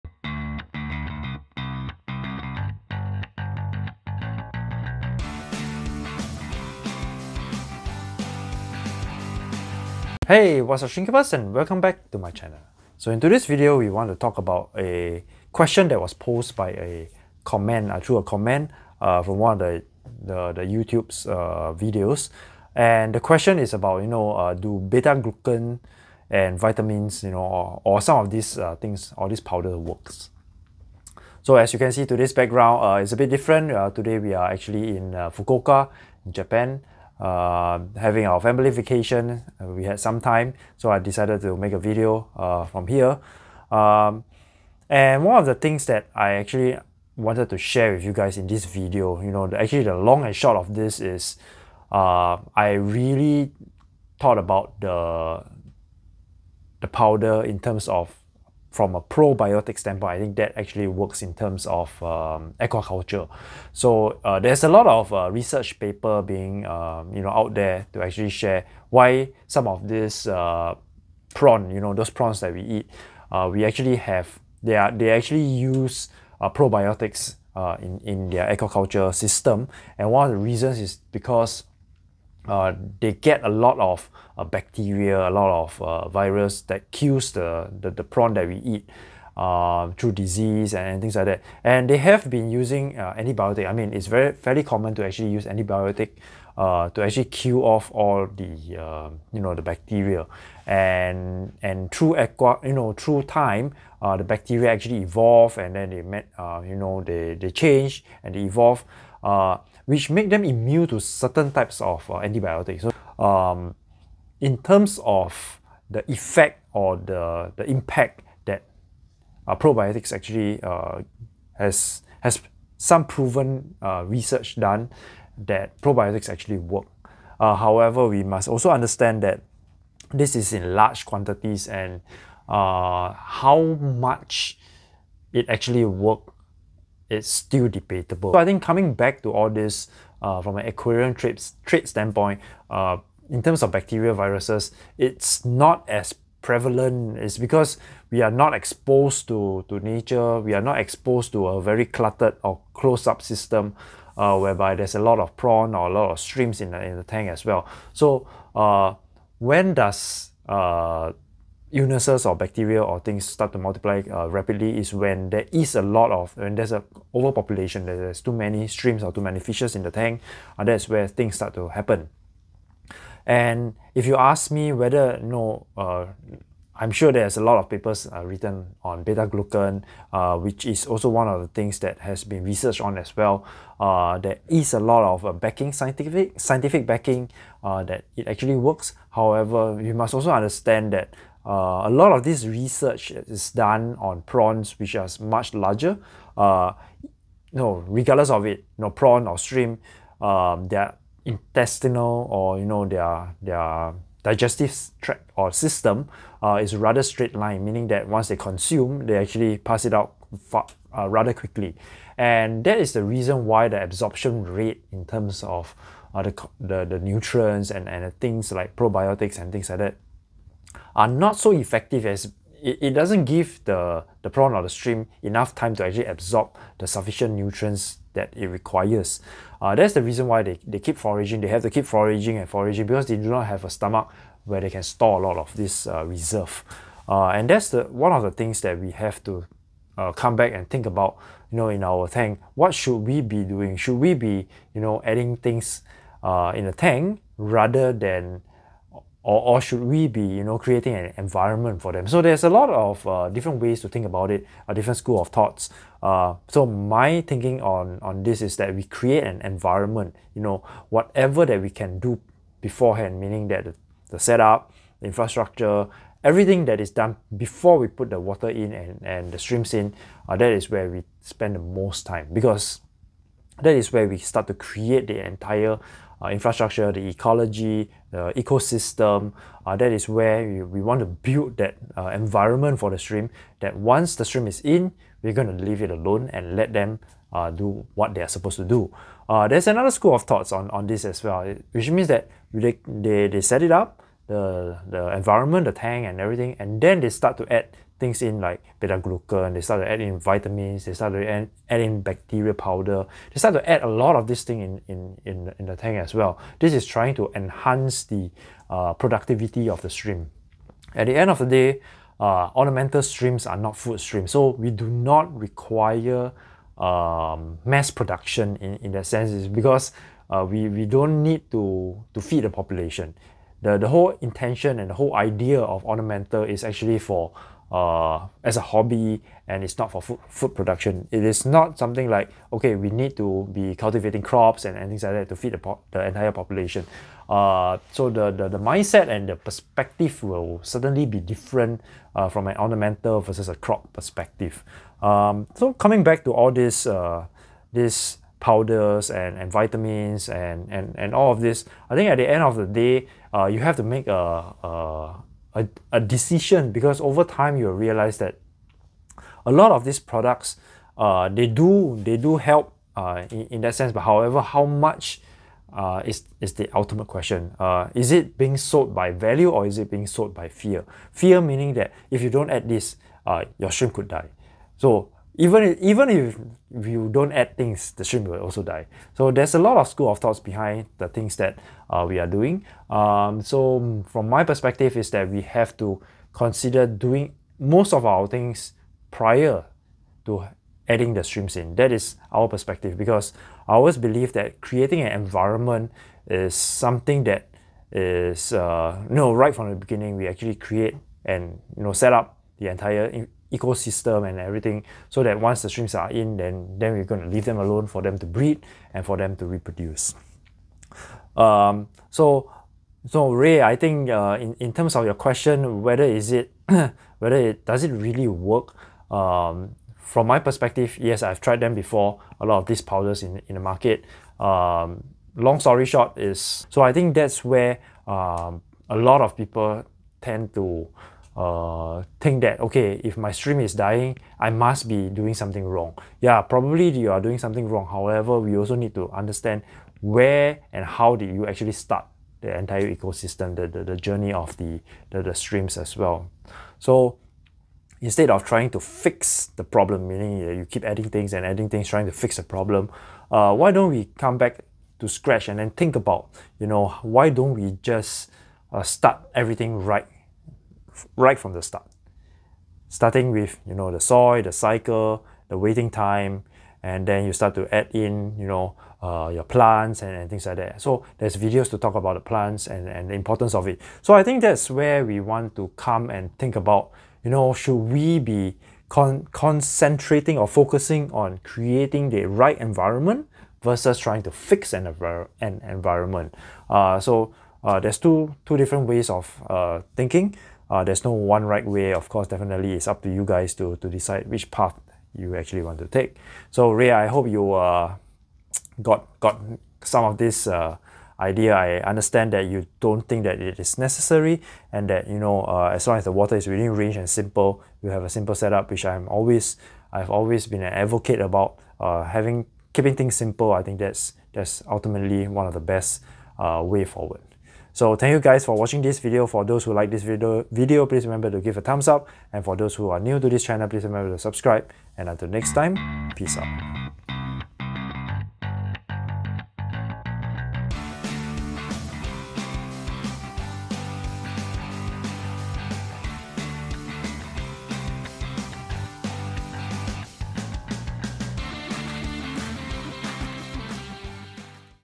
Effects of Additives
Adding more things vs setting it up first time right? In this video we will share with you the effects of adding additives.

0:00 Intro and shoutout
0:50 Getting into probiotics
4:50 why it matters
9:00 closing comments